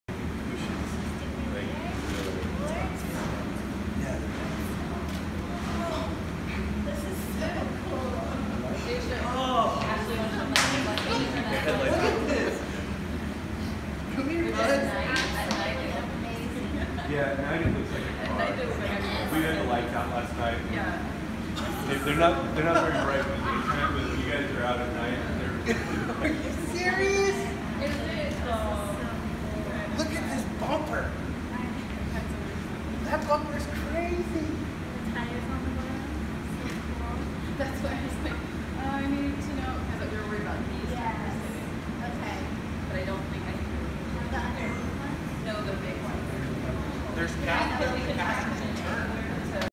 Turlock boy gifted with Magic wheelchair by Lathrop High team
Eight-year-old Turlock boy named Cash receives a RC version of a monster truck that was handmade by Kenneth Scott Myers and students of be.Next at Lathrop High School.